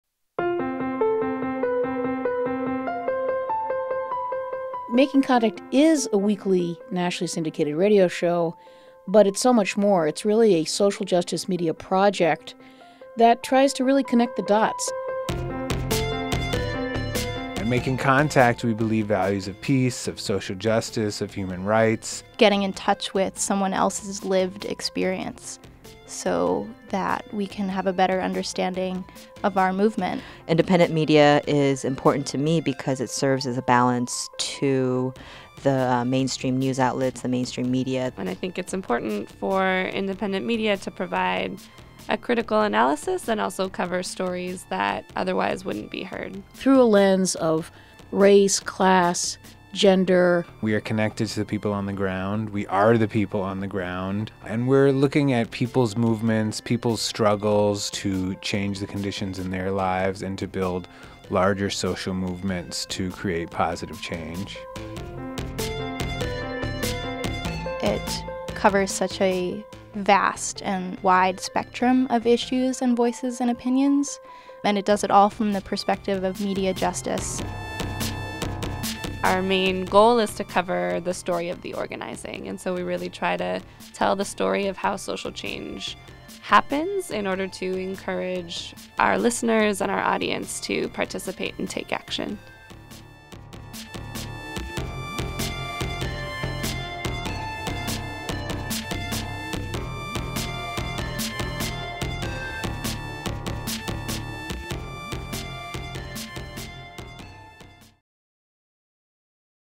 What is Making Contact?
Making Contact produces media that analyzes critical issues and showcases grassroots solutions, in order to inspire audiences to action.

Movements begin with story-telling. Making Contact is the ­only nationally syndicated radio series that covers the stories of change-makers from the ground up.

We train independent journalists and community members in radio skills, improving popular access to the media. Through our community collaborations, National Radio Project increases the media capacity of community groups and helps amplify the voices of those working and organizing for social justice.

Making Contact is committed to in-depth critical analysis that goes beyond the breaking news.We tell the stories of the 99%- and air the voices of change-makers from the bottom up.

Your support grows the media of the 99%, and makes sure that listeners on 140 stations across the country and the world are inspired by the voices and visions of grassroots movement for social change.